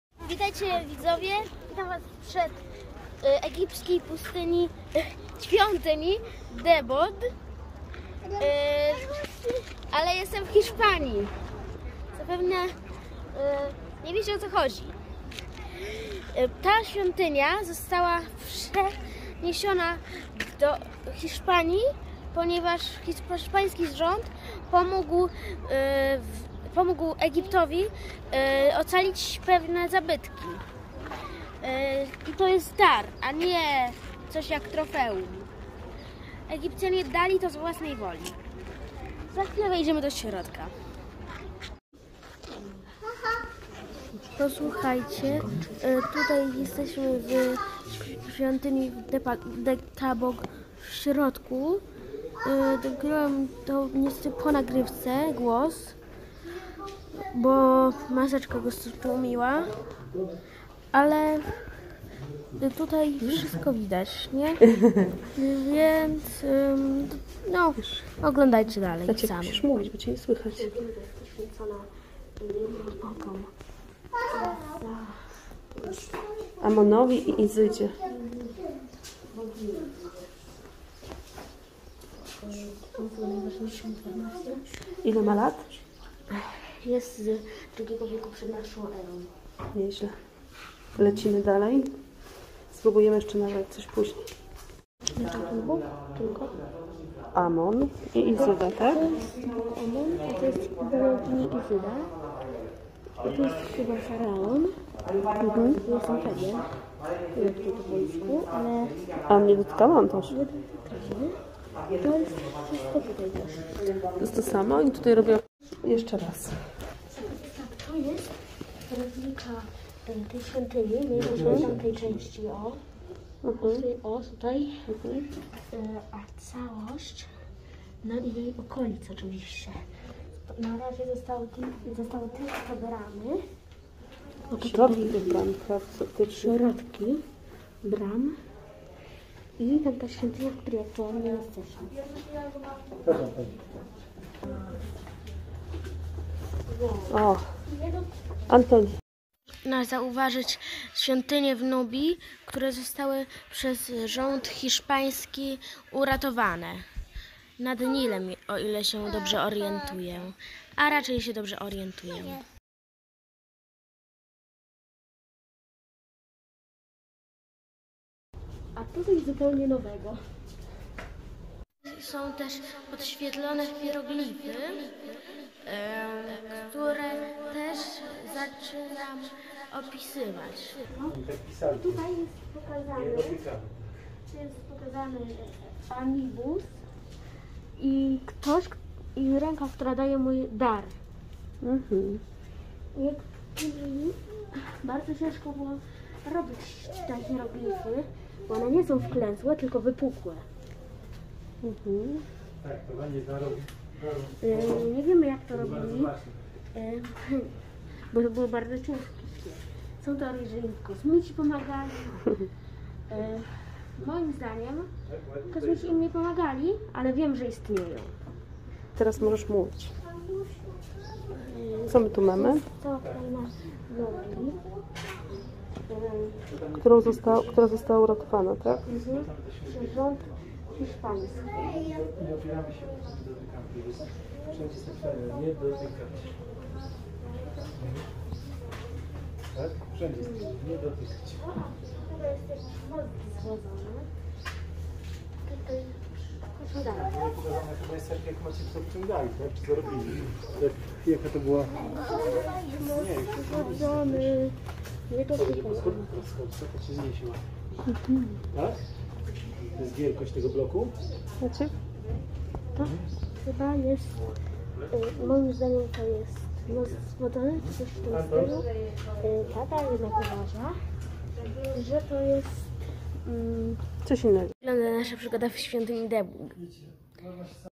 świątynia Debod Madryt - z zewnątrz i wewnątrz
Hieroglify! Makiety! i wiele więcej w świątyni Debod